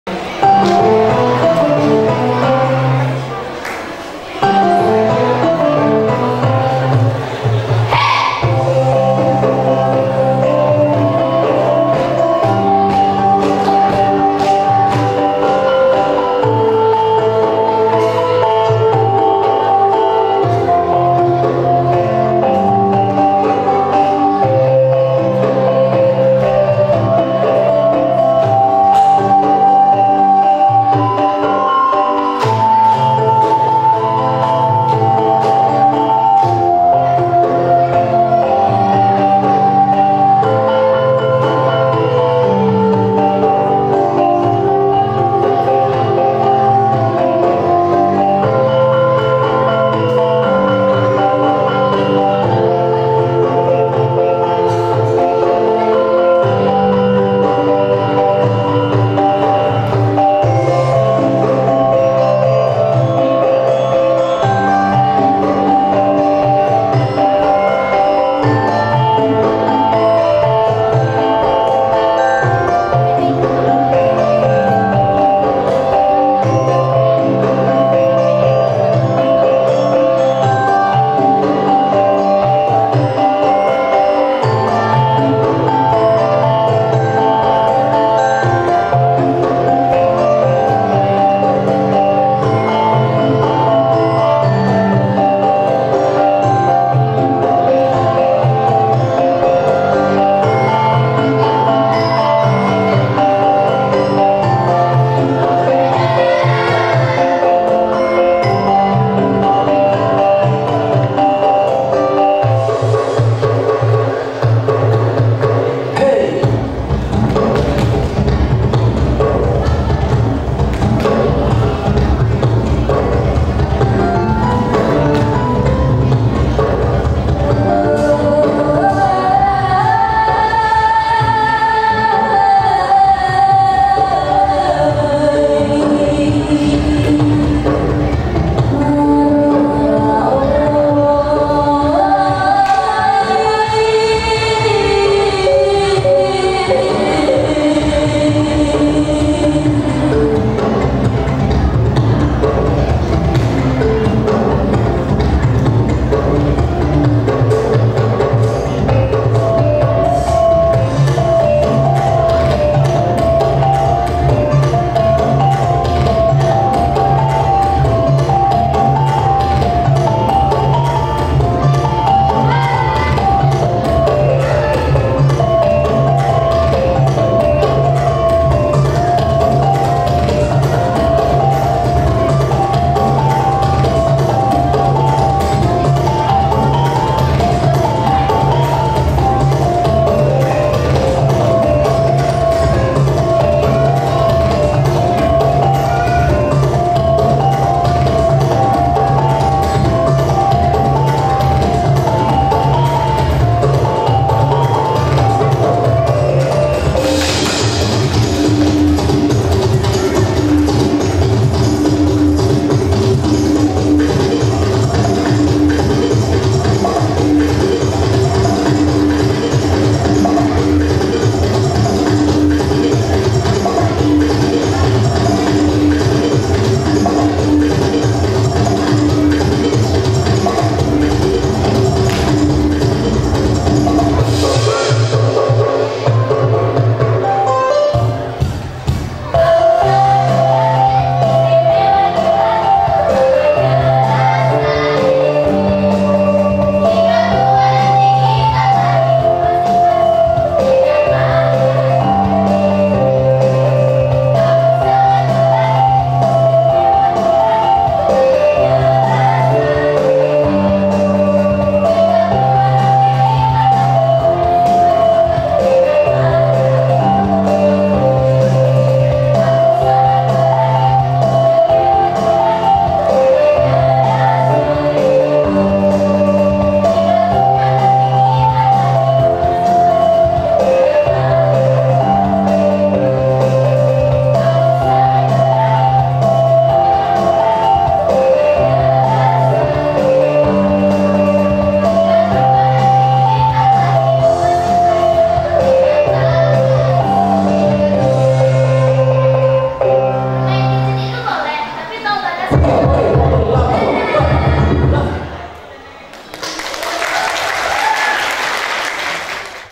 Gadget - SMPN 7 Padang || UAS Tari Pendidikan - Sendratasik UNP Ak.2021
Ujian Akhir Semester Juli - Desember  2023
Mata Kuliah : Tari Pendidikan
Dosen : Prof. Dr. Fuji Astuti, M.Hum
Mahasiswa : Sendratasik Angkatan 2021

Tempat Ujian : Auditorium FBS UNP lantai 4 Gedung Laboratorium FBS UNP
12 Desember 2023

Kamerawan : Mamek
Edit Video : J-Two Media
Sound & Lampu : Narji